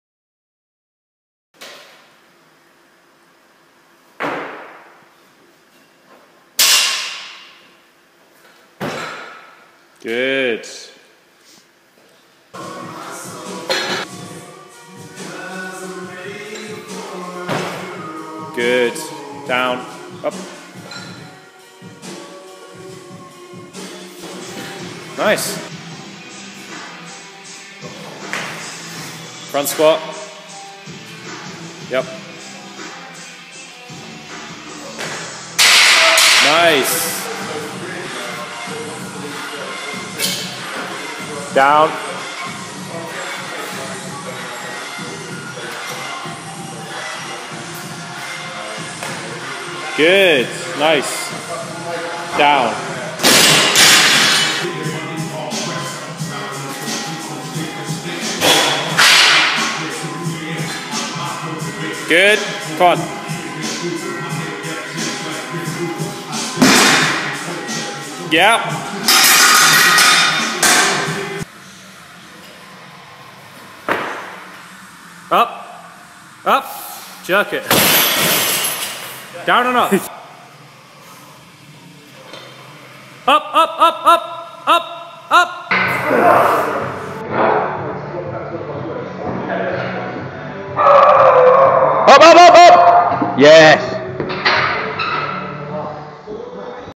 This Week at PPG WLC - 2.12.13
Regular Michael making some pbs and big lifts. Megan returning to the club after a hiatus and getting back into things nicely. A 5kg pb for Paul on the back squat